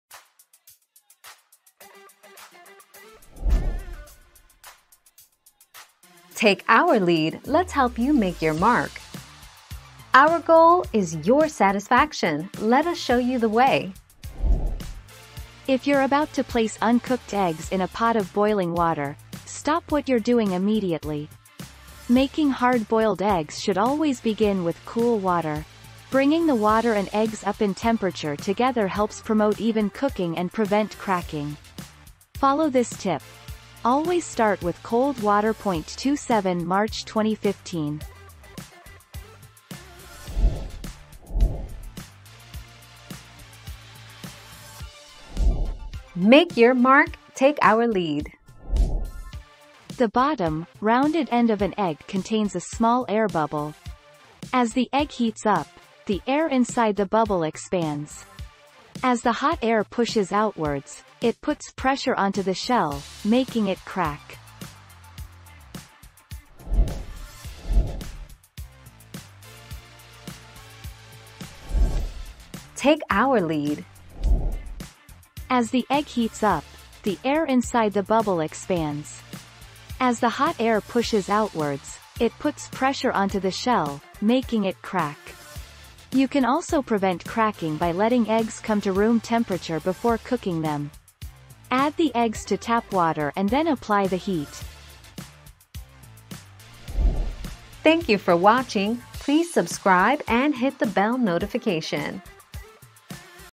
How do I keep my eggs from cracking when I boil them?
00:00 - How do I keep my eggs from cracking when I boil them?
00:42 - Why do eggs crack when put in boiling water?
01:08 - Why do eggs crack when boiling them?

Laura S. Harris (2021, April 19.) How do I keep my eggs from cracking when I boil them?
    AskAbout.video/articles/How-do-I-keep-my-eggs-from-cracking-when-I-boil-them-237882